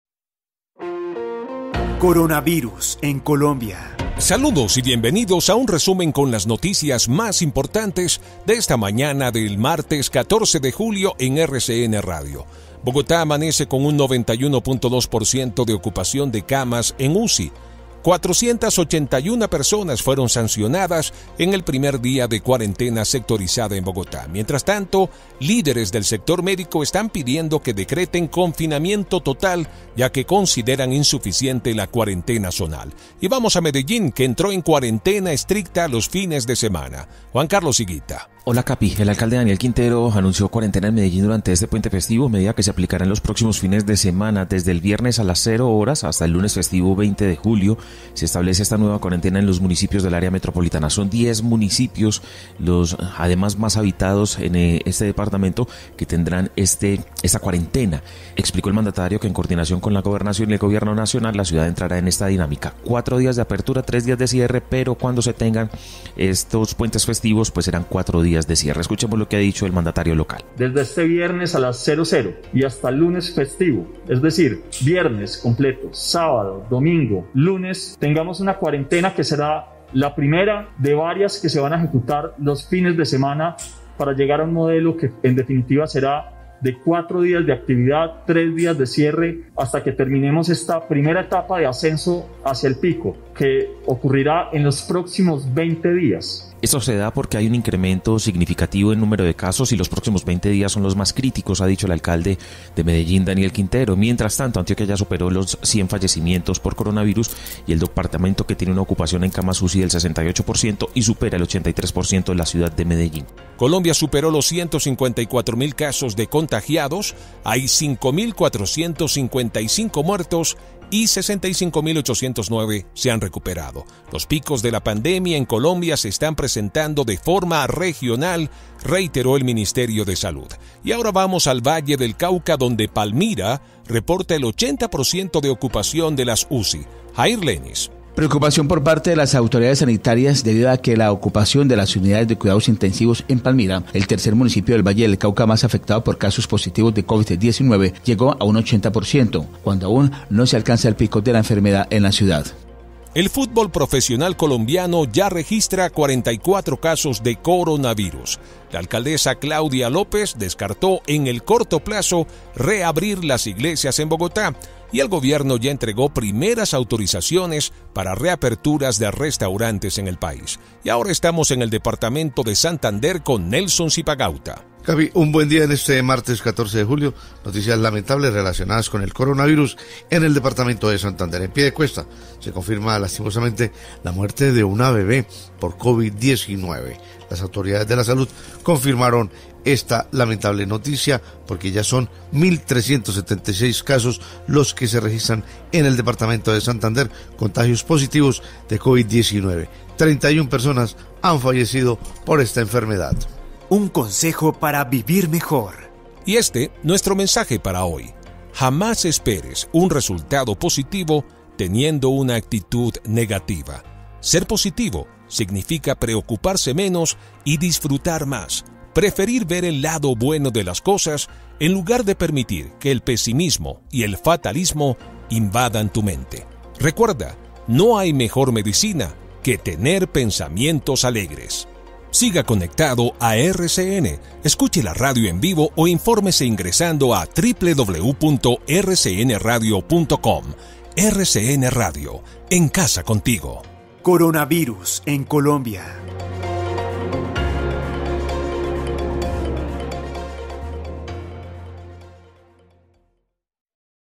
Bogotá llegó al 91,2% de ocupación de UCI
RCN Radio - Actualización de noticias Julio 14 de 2020